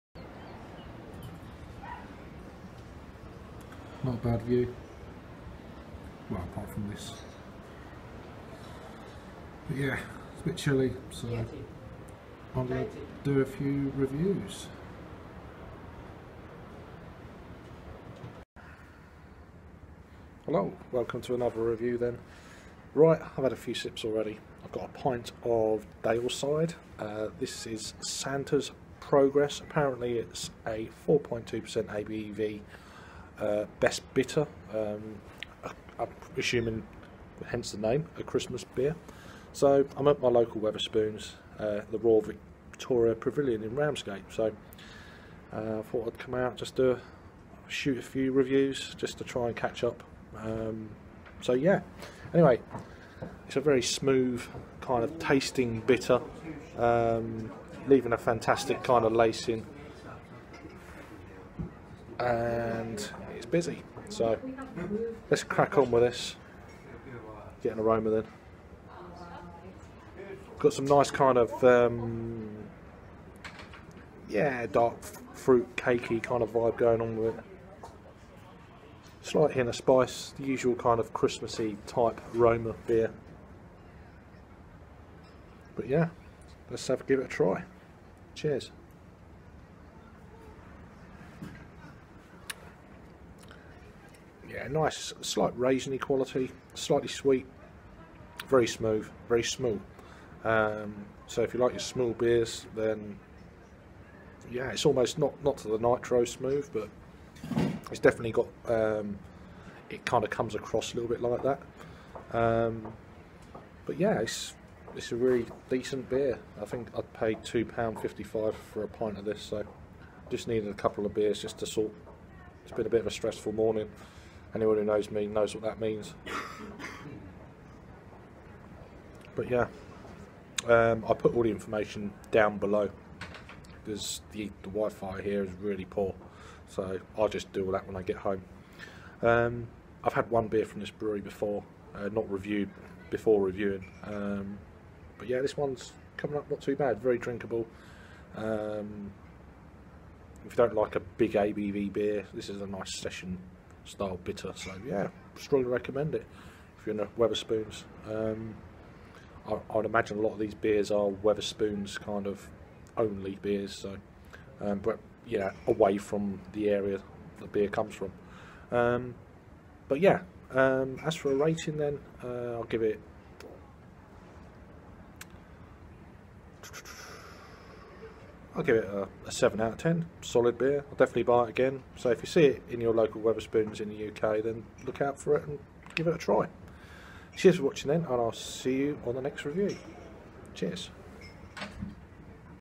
#788 Daleside Brewery | Santa’s Progress 4.2%ABV Bitter (British Ale)
Brewery - Daleside Brewery
Location - Harrogate,North Yorkshire,UK 🇬🇧
Beer - Santa’s Progress
Style - English Bitter
ABV - 4.2%
IBUs - Not Available.
Reviewed on - 29th November 2017.
Commercial Description - Santa’s Progress is a well-balanced bitter, Burnished gold in colour with fruity aromas and a warming malty finish.

Daleside Brewery: - Quintessentially British
National Award Winning Brewery from Harrogate in Yorkshire.
Daleside Brewery has been trading since the mid 1980's. Since then our beers have gone on to win National and Regional Awards. We always use highest quality malt and freshest whole hops in all our beers.